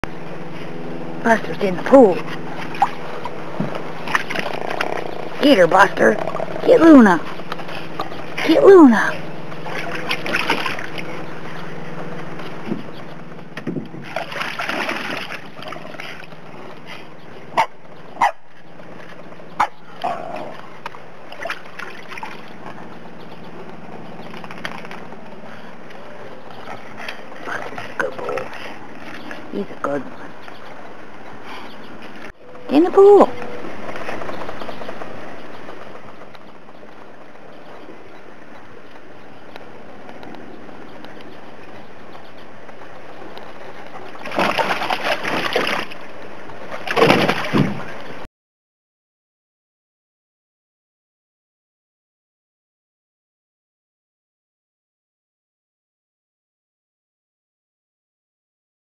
Boston Terrier Pool Guard
Buster (the pool guard) is privy to information (sharks spotted?) not readily available to the rest of us it seems. He takes his job very seriously, and is determined that no one comes to harm under his watch...